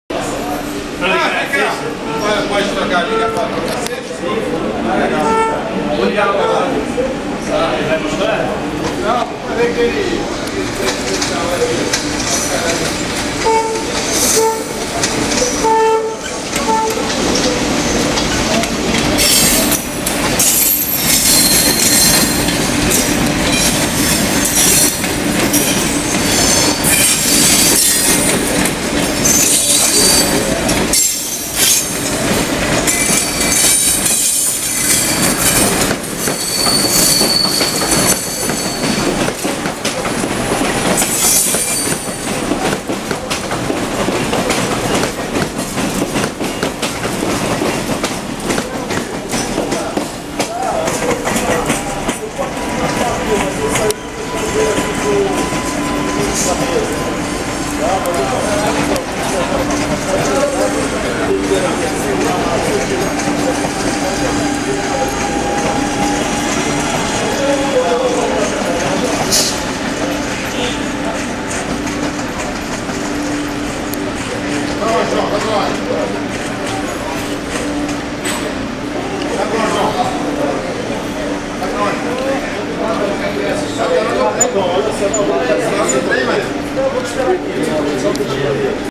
Movimentação de trens na Central do Brasil - Parte 2
Trens no pátio da estação terminal Central do Brasil.